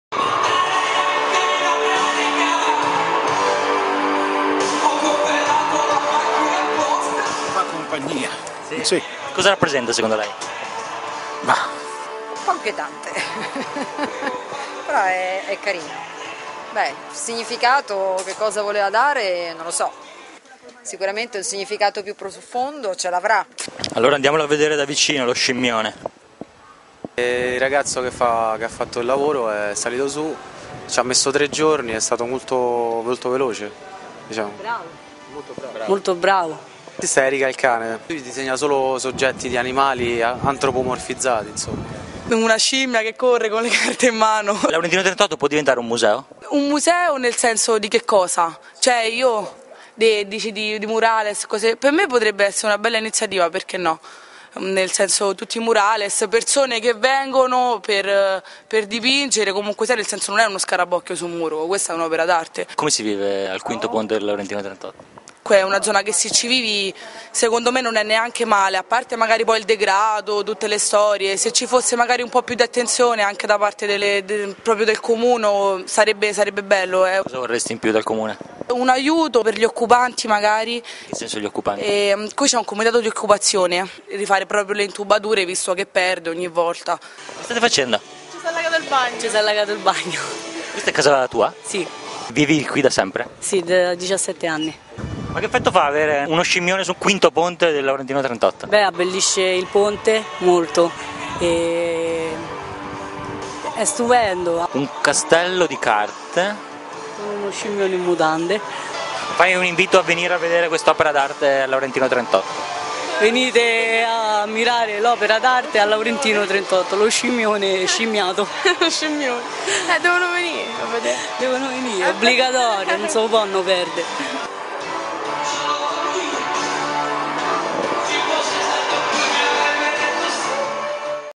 Lo scimmione del Laurentino 38
Roma. Su un palazzone del Laurentino 38 e' comparso uno scimmione che si muove tra dei castelli di carte. E' un opera dell'artista ericailcane realizzato con l'associazione pontedincontro onlus. Siamo andati a vedere. Servizio di Fabio Butera.